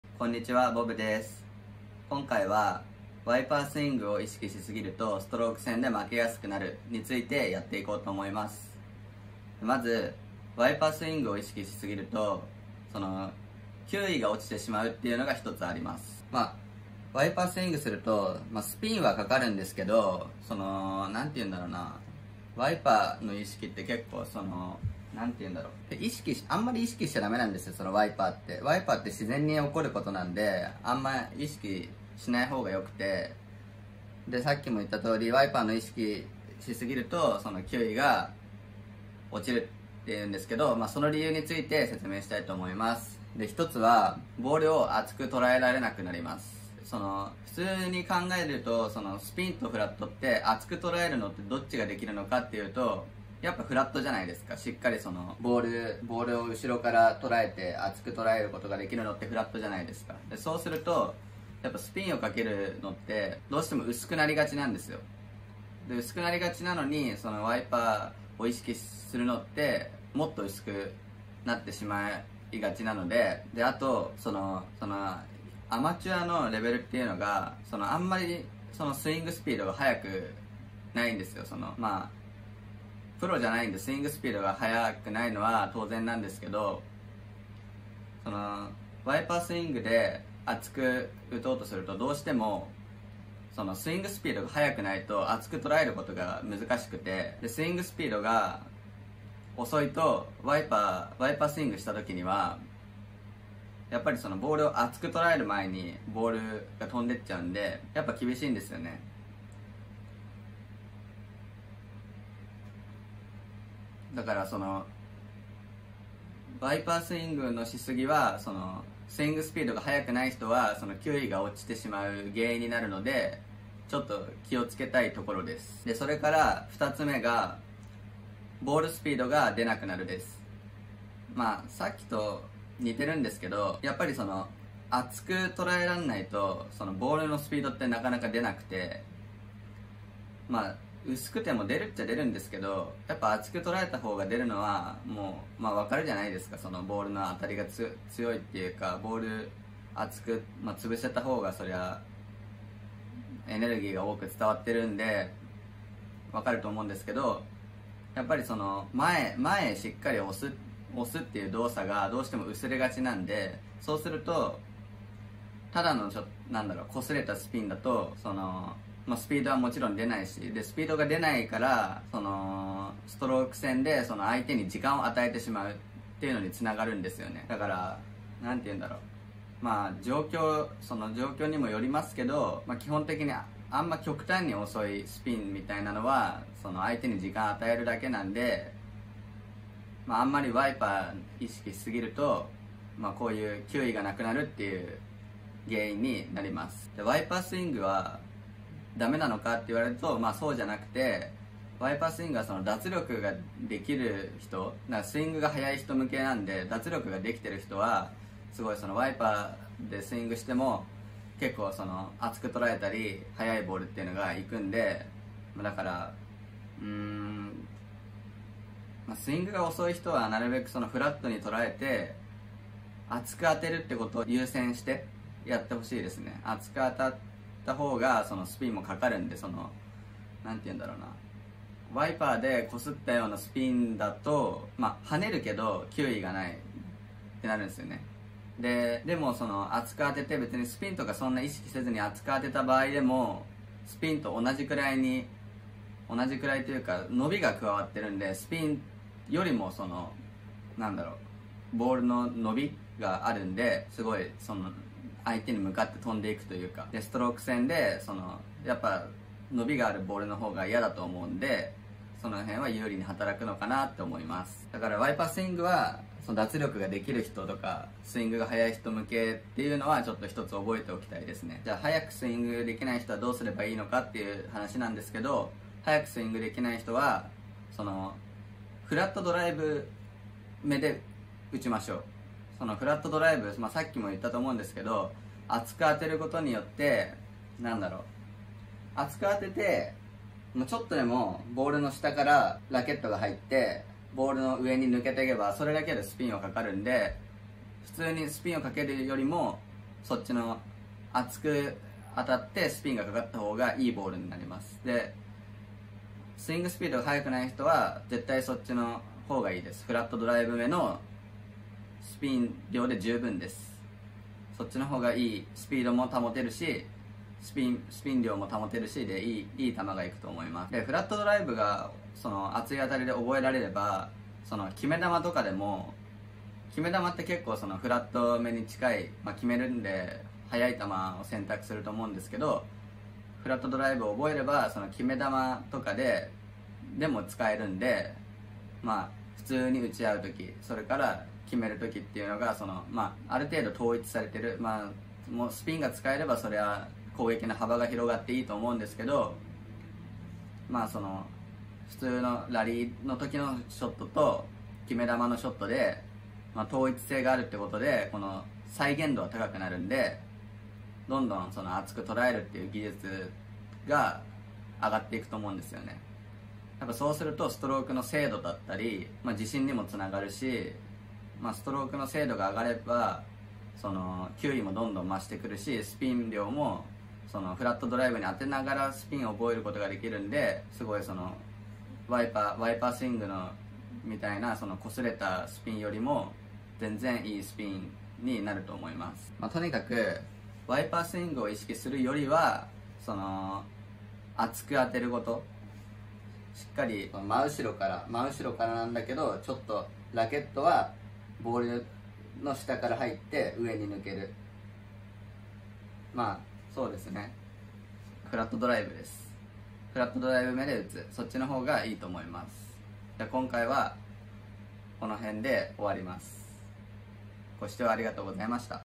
【ストローカー必見】ワイパースイングだとストローク戦で負けやすくなる！？
ぼぶです。
今回は、ワイパースイングだとストローク戦で負けやすいについて解説しました。
一般プレーヤーの視点でストローク戦を有利に戦える方法を提案しているので、参考になれば嬉しいです。